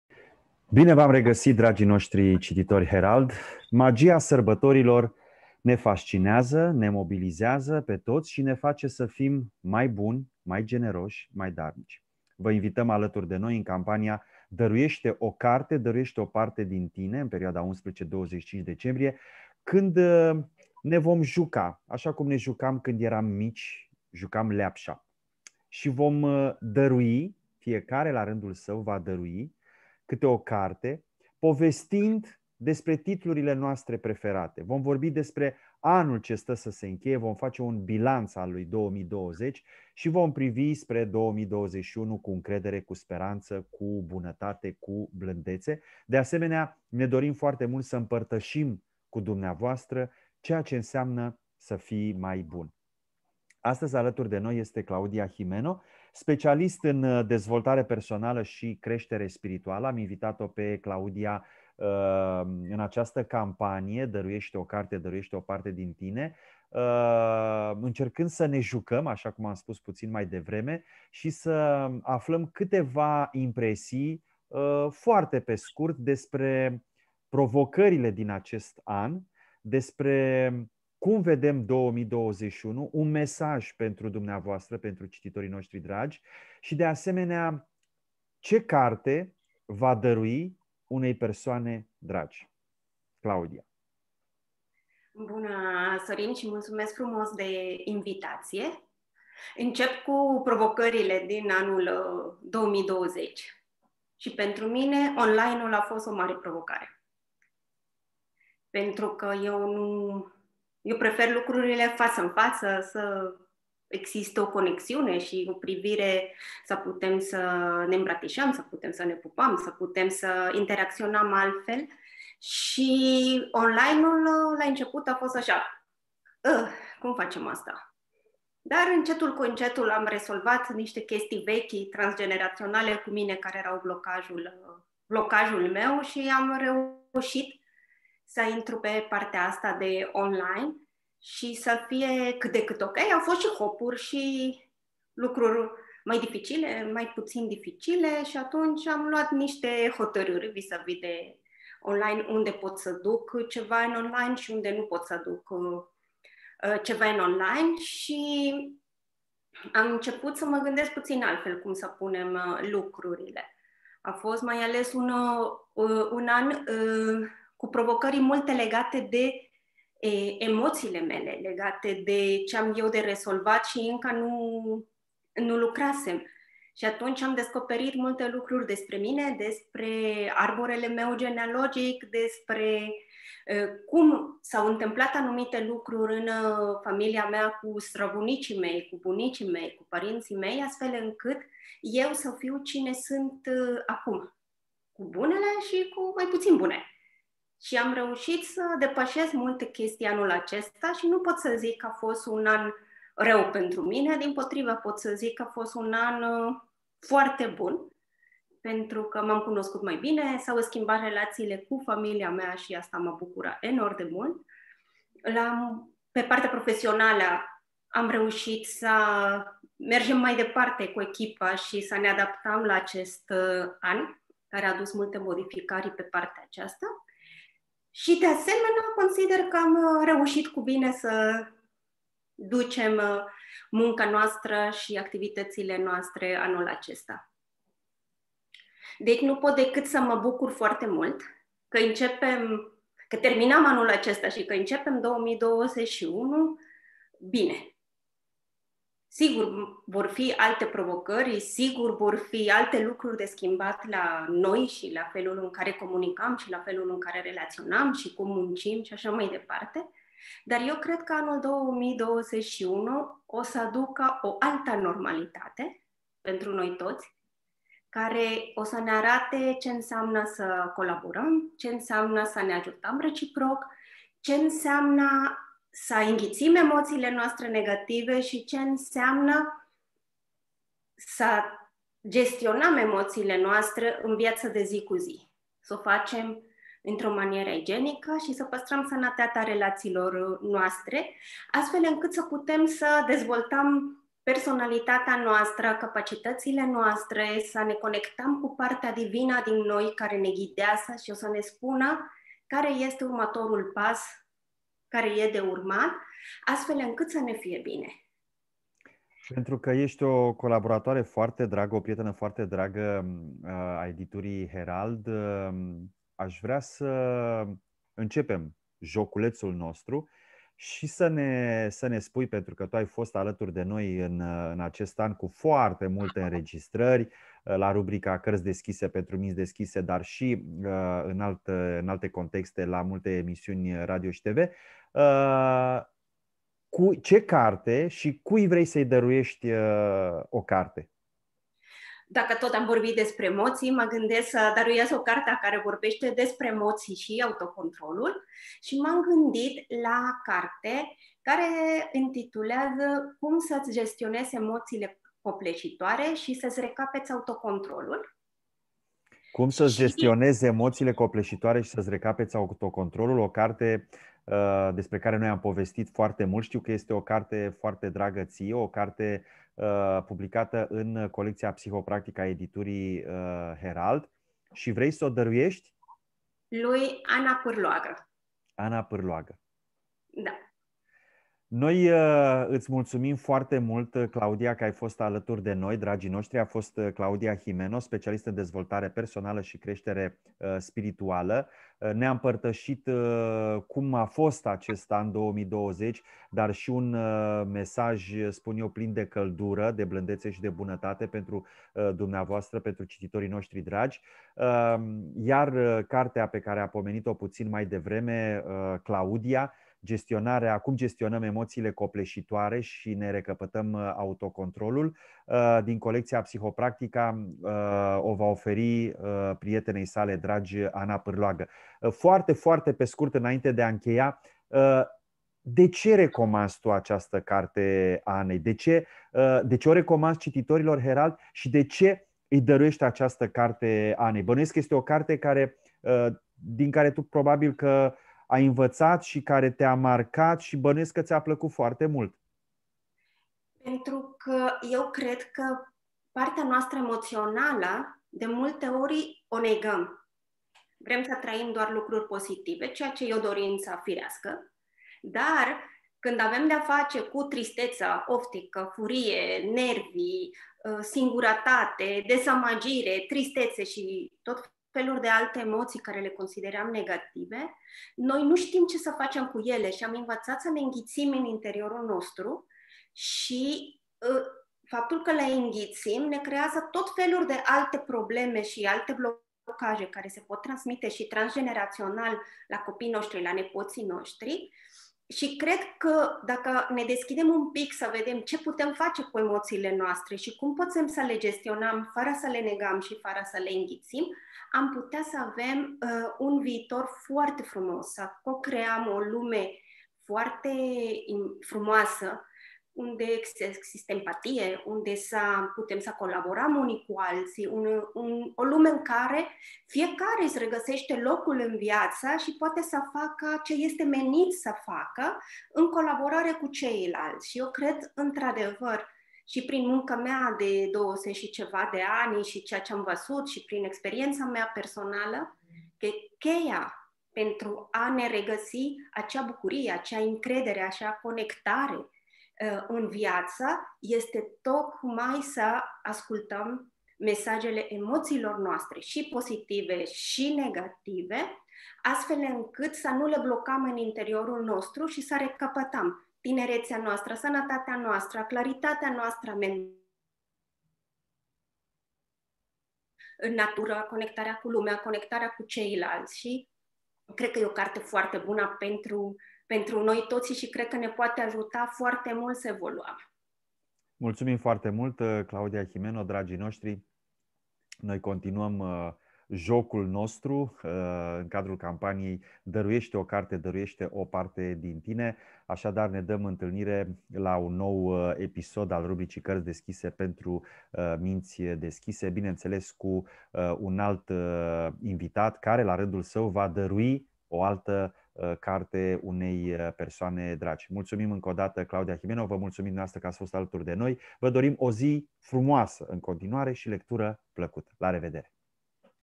Claudia Jimeno - Cum să-ți gestionezi emoțiile copleșitoare și să-ți recapeți autocontrolul
Claudia Jimeno Beltran este specialist în dezvoltare personală și creștere spirituală, direcții în care și-a dedicat întreaga viață și anii de studiu. În această direcție, în anul 2001 a înființat Centrul de Dezvoltare Personală AmaneSer, este practicant al Metodei ESPERE®, trainer și moderator pentru grupuri de comunicare și relaționare interpersonală eficientă și grupuri de dezvoltare personală.

Astăzi în cadrul unui dialog cu Sorin Lucaci (PR Herald), Claudia a recomandat cartea „Cum să-ți gestionezi emoțiile copleșitoare și să-ți recapeți autocontrolul. Ghidul practic de terapie comportamentală dialectică", și dăruit-o lui Ana Parloaga.

Dezvoltată de Marsha Linehan pentru a trata tulburarea de personalitate borderline, terapia comportamentală dialectică (TCD) s-a dovedit o terapie eficientă pentru o multitudine de probleme de sănătate mintală, iar cercetările i-au dovedit eficiența în special în gestionarea emoțiilor copleșitoare și a suferinței psihologice, fără a pierde controlul și a acționa distructiv. Pentru a beneficia de efectele pozitive ale acestei terapii, din cartea de față veți învăța cum să vă dezvoltați abilități în patru mari domenii psihologice:
- toleranța la stres și suferință
- prezență conștiență (mindfulness)
- gestionarea emoțiilor
- eficiența interpersonală

📖 „Cum să-ți gestionezi emoțiile copleșitoare și să-ți recapeți autocontrolul. Ghidul practic de terapie comportamentală dialectică", McKay, Brantley, Wood, colecția Psihopractica